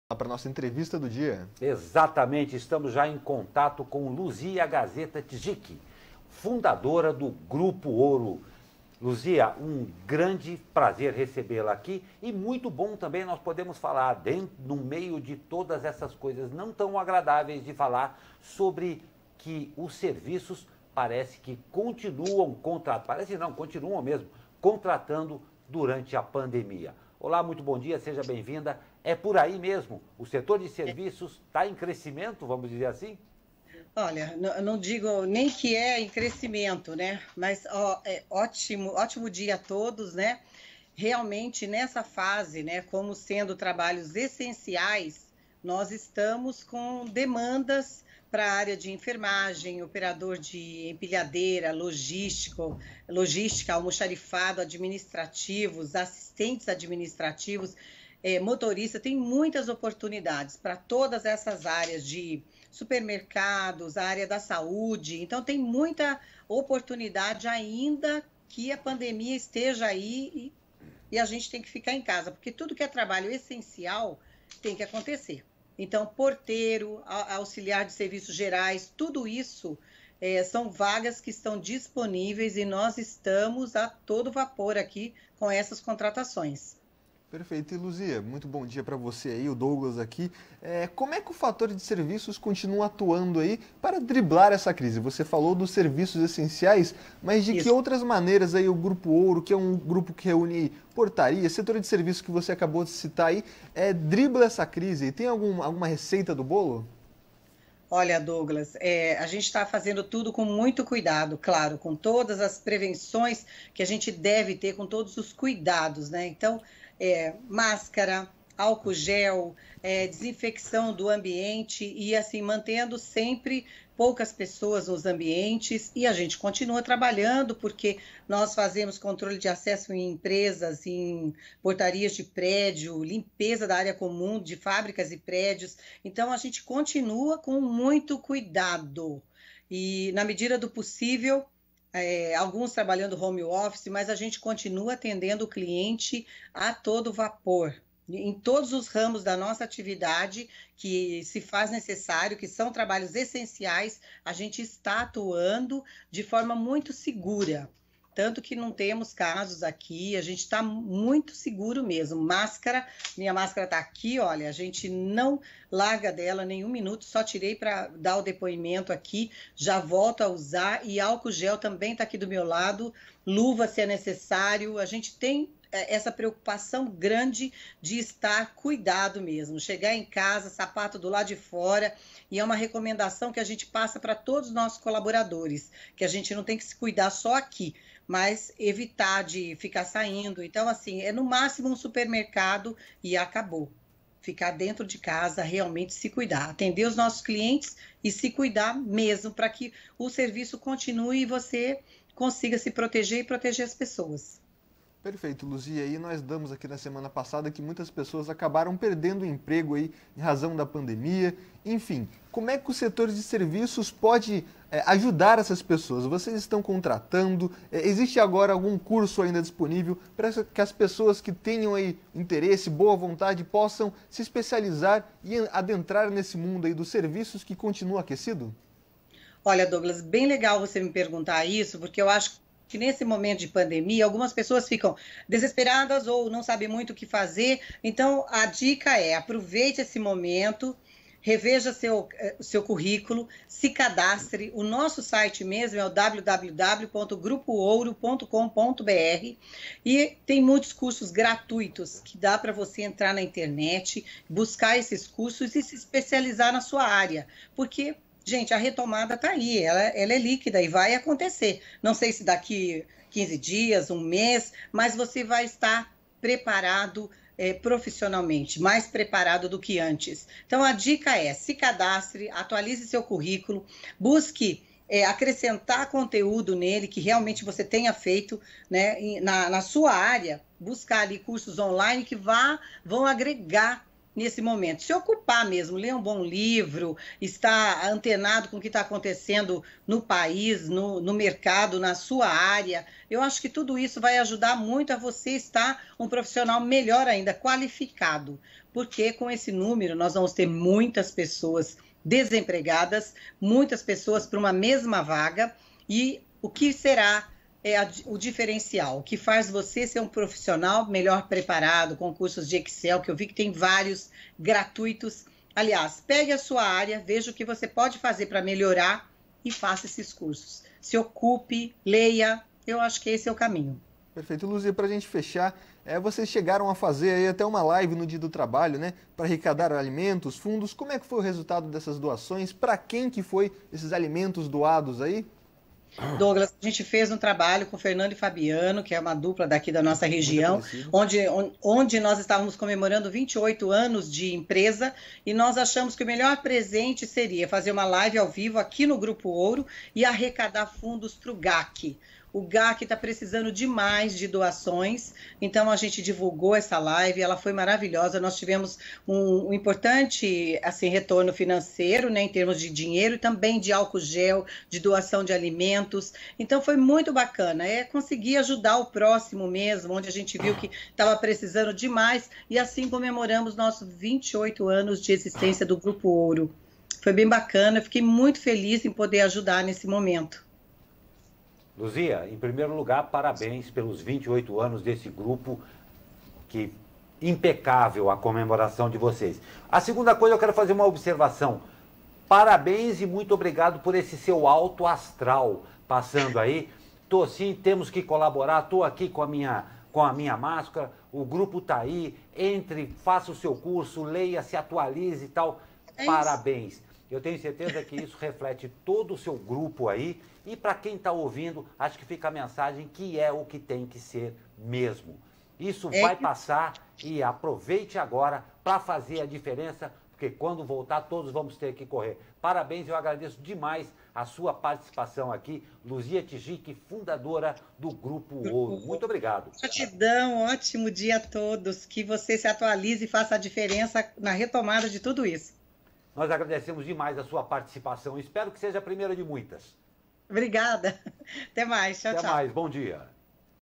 Luzia Gasetta Tschizik, fundadora do Grupo Ourho
Confira a entrevista completa com Luzia Gasetta Tschizik, fundadora do Grupo Ourho. Ela veio falar sobre o setor de serviços, que continua contratando durante a pandemia.